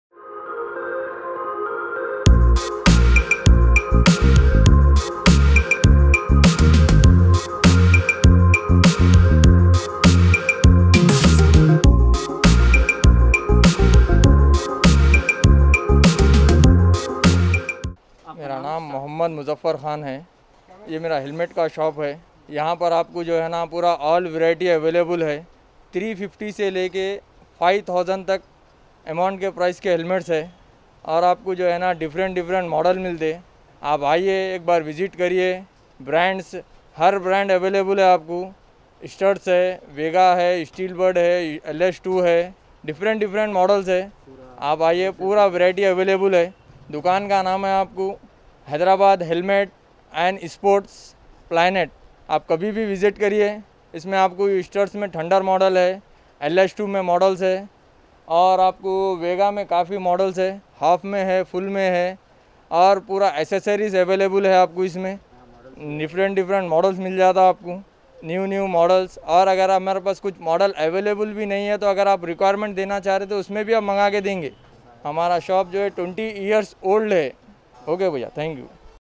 Branded Helmets at Wholesale Price || Hyderabad Sports & Fitness Planet || Hybiz tv
for bike riders. If you can't prevent a crash or impact, but you know it will occur, a helmet can prevent or minimize injury to the head and brain. Motorcycle helmets reduce the impulse on the head in case of a crash, by softening the impact. Two-wheeler helmets come in different types based on ride style, nature of the ride (long or short) and other requirements.